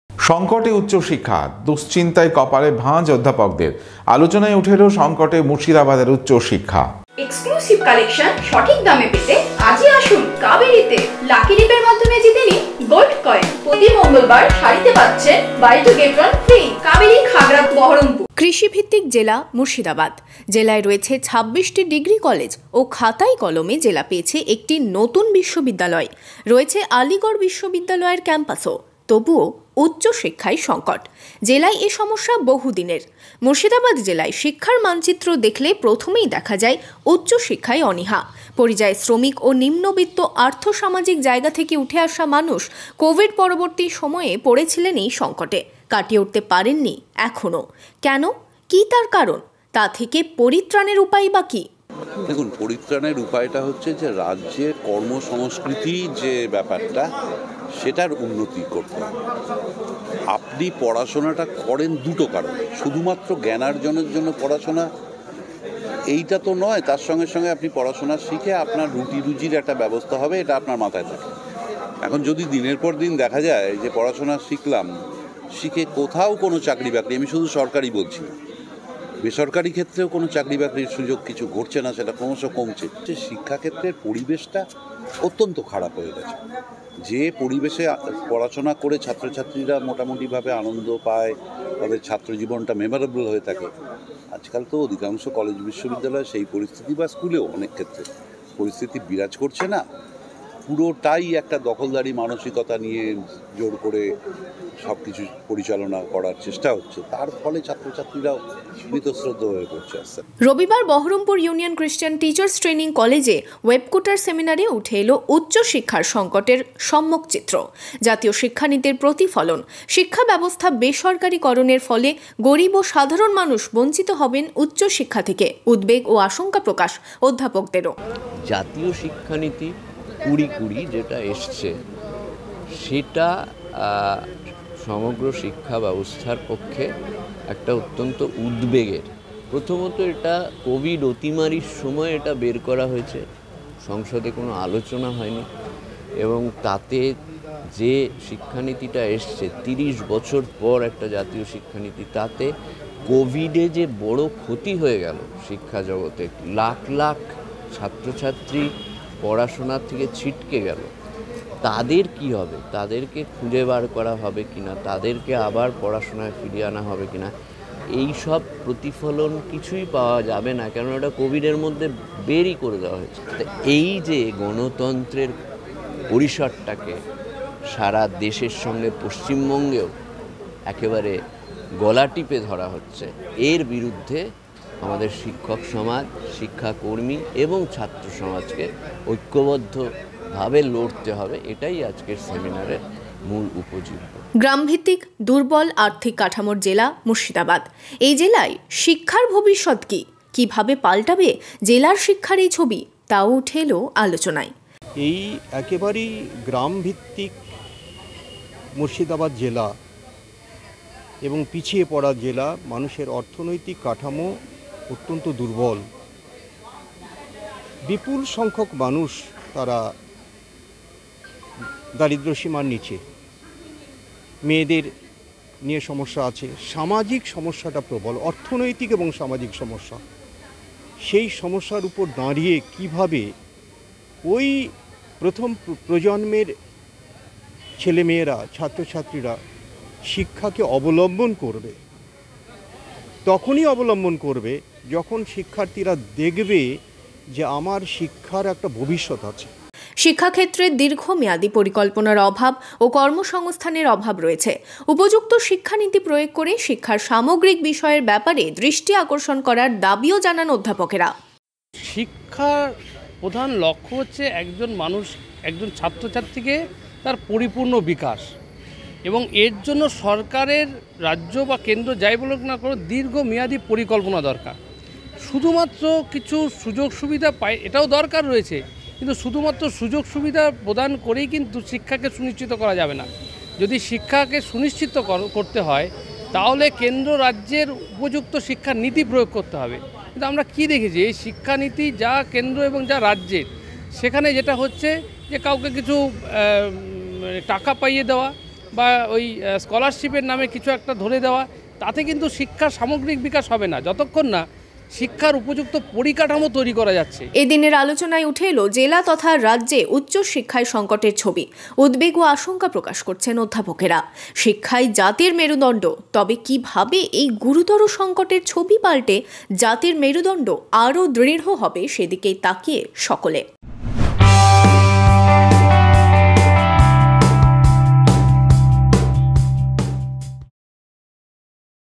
Murshidabad Education: উচ্চশিক্ষার অবস্থা নিয়ে দুশ্চিন্তা অধ্যাপকদের
উচ্চশিক্ষার অবস্থা নিয়ে দুশ্চিন্তা অধ্যাপকদের আলোচনায়